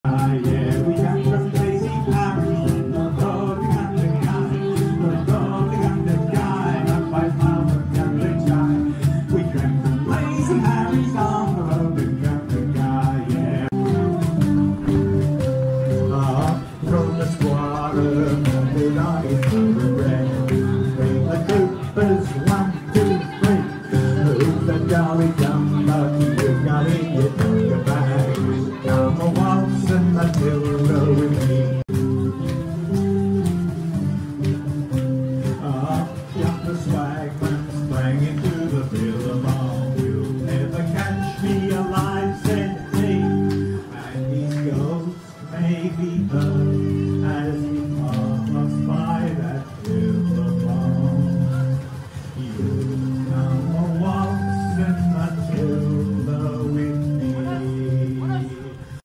Chord-eaux - Aussie Songs at Nan Tien Temple
Chord-eaux played Aussie traditional folk songs as part of the multi-cultural performances at the Nan Tien Temple Chinese New Year celebrations in February 2018